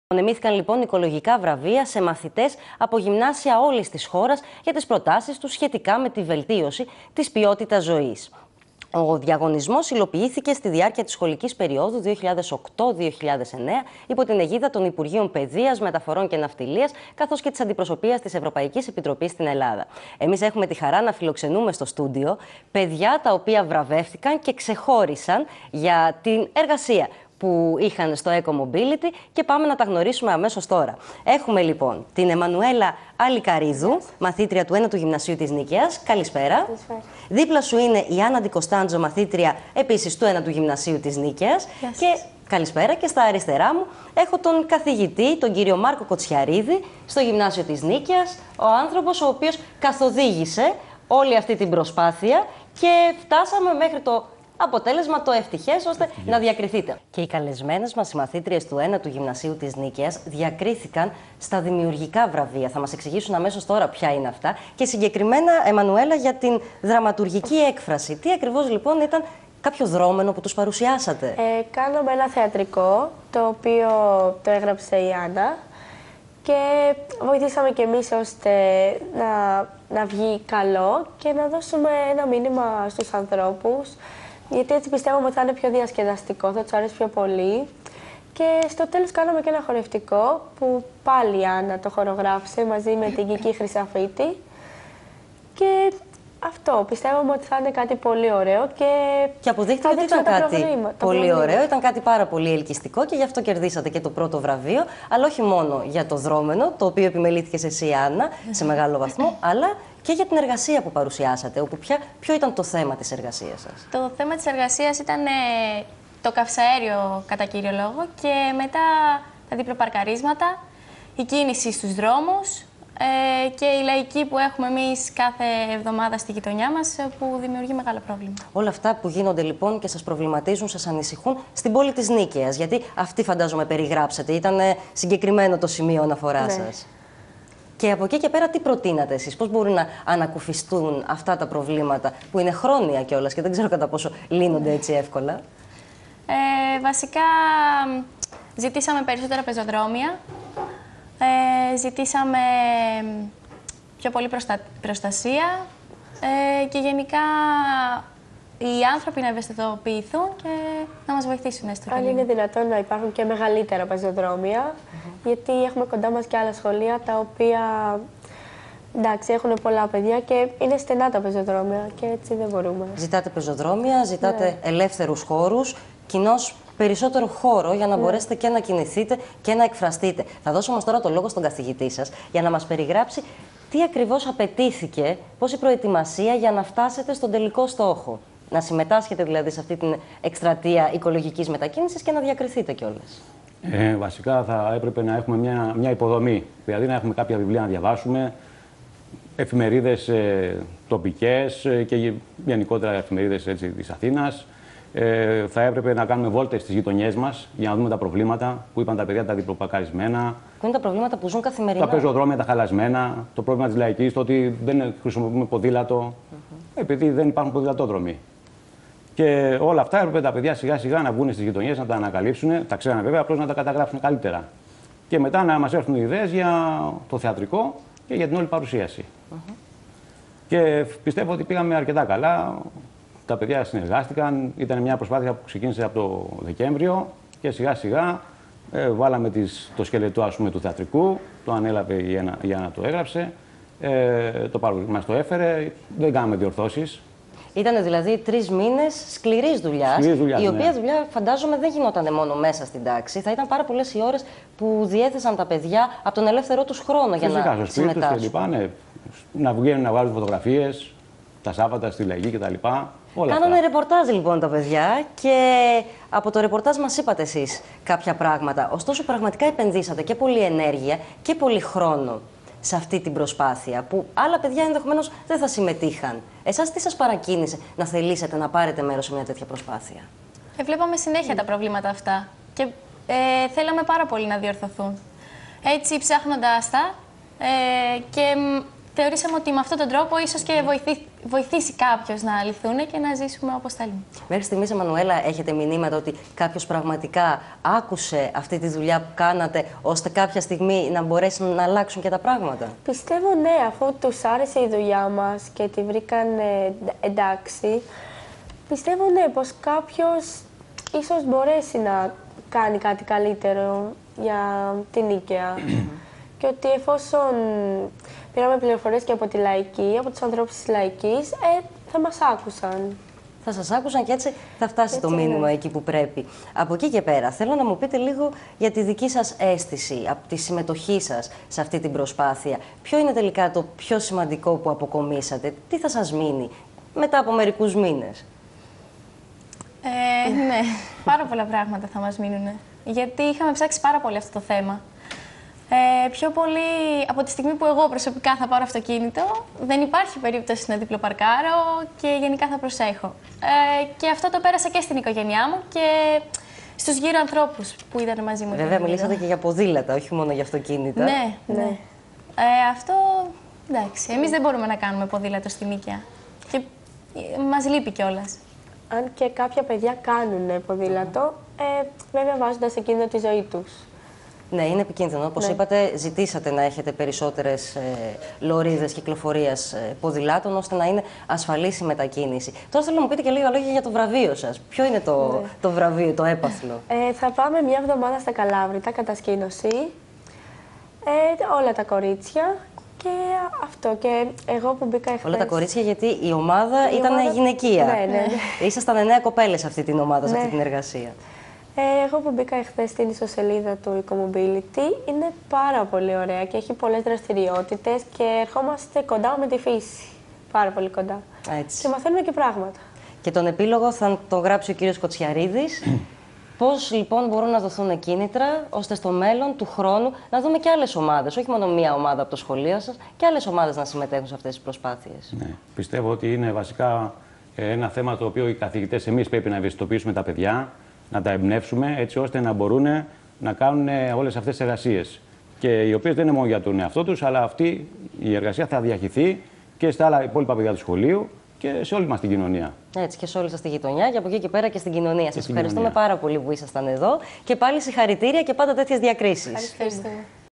ΒΡΑΒΕΙΟ 9ου ΝΙΚΑΙΑΣ..avi
Η  Περιβαλλοντική  Ομάδα του  9ου  Γυμνασίου   Νικαίας   απέσπασε  το  1ο  βραβείο  για τη   Δραματουργική   Δημιουργία  της   στην  εκστρατεία  ECOMOBILITY    της   μ.κ.ο.  ECOCITY.  Οι υπεύθυνοι    της   ΕΡΤ  μας   κάλεσαν   στο  ραδιομέγαρο  για  να   παρουσιάσουμε   τι   δημιουργήσαμε.